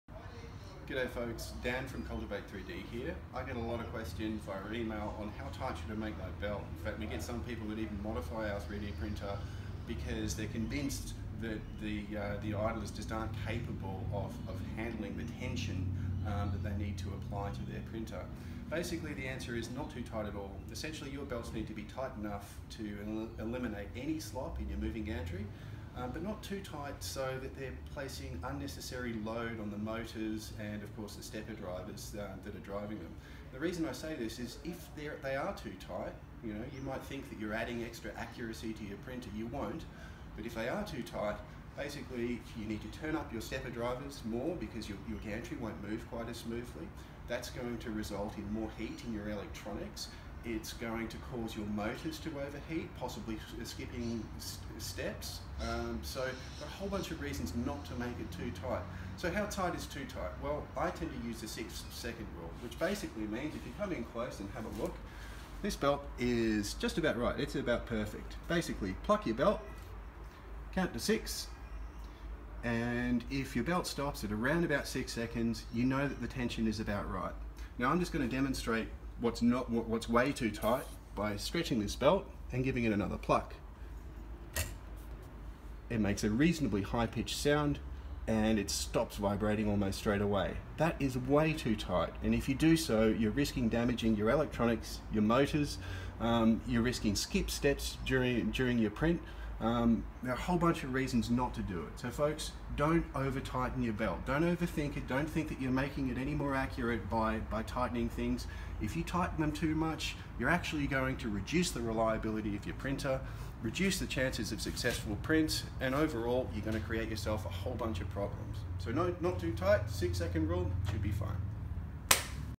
3D Printer. How tight should my belts be?
Getting the right belt tension is the difference between a cool running, accurate 3D printer that doesn't skip steps and an over driven machine which skips steps with the slightest of Z obstructions.  Follow my 6 second rule and don't think that super tight belts gives you an more accuracy than sufficiently taught ones.